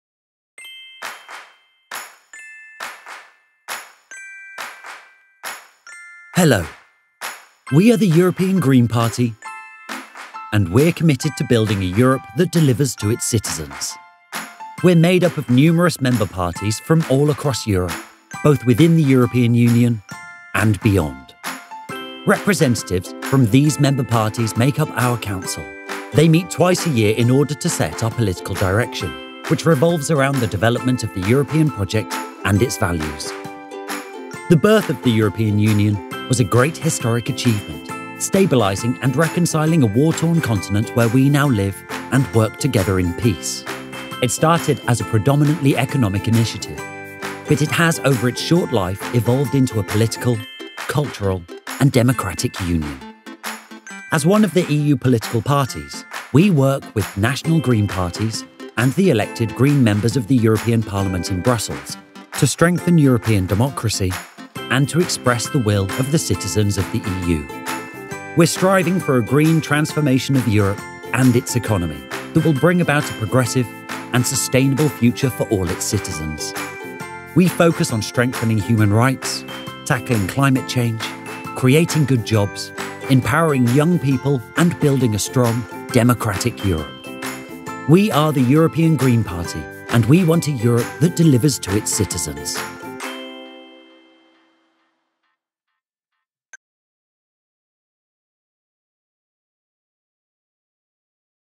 Ευρωπαϊκό Πράσινο Κόμμα | ΕL
Γεια σας. Είμαστε το Ευρωπαϊκό Πράσινο Κόμμα και έχουμε δεσμευτεί στη δημιουργία μιας Ευρώπης που προσφέρει στους πολίτες της.

Αποτελούμαστε από πολλά κόμματα μέλη από όλη την Ευρώπη, τόσο από την Ευρωπαϊκή Ένωση όσο και από άλλες χώρες.

Οι εκπρόσωποι αυτών των κομμάτων μελών απαρτίζουν το Συμβούλιό μας. Συναντιούνται δύο φορές τον χρόνο προκειμένου να ορίσουν την πολιτική μας κατεύθυνση η οποία στρέφεται γύρω από το ευρωπαϊκό σχέδιο και τις αξίες του.

Η γέννηση της Ευρωπαϊκής Ένωσης ήταν ένα σπουδαίο ιστορικό επίτευγμα το οποίο σταθεροποίησε και συμφιλίωσε μια ήπειρο που είχε πληγεί από τον πόλεμο, την ήπειρο στην οποία σήμερα ζούμε και εργαζόμαστε μαζί ειρηνικά. Ξεκίνησε ως μια πρωτίστως οικονομική πρωτοβουλία, αλλά στη σύντομη ζωή της εξελίχθηκε σε μια πολιτική, πολιτιστική και δημοκρατική ένωση.

Ως ένα από τα πολιτικά κόμματα της ΕΕ, συνεργαζόμαστε με τα εθνικά κόμματα Πρασίνων και τα εκλεγμένα μέλη των Πρασίνων στο Ευρωπαϊκό Κοινοβούλιο στις Βρυξέλλες, για να ενισχύουμε την ευρωπαϊκή δημοκρατία και να εκφράζουμε τη βούληση των πολιτών της ΕΕ. Επιδιώκουμε μια Πράσινη μεταμόρφωση της Ευρώπης και της οικονομίας της η οποία θα δημιουργήσει ένα προοδευτικό και αειφόρο μέλλον για όλους τους πολίτες της.

Εστιάζουμε στην ενίσχυση των ανθρωπίνων δικαιωμάτων, την αντιμετώπιση της κλιματικής αλλαγής, τη δημιουργία καλών θέσεων εργασίας, την ενδυνάμωση των νέων ανθρώπων και το χτίσιμο μιας ισχυρής, δημοκρατικής Ευρώπης.

Είμαστε το Ευρωπαϊκό Πράσινο Κόμμα και θέλουμε μια Ευρώπη που προσφέρει στους πολίτες της.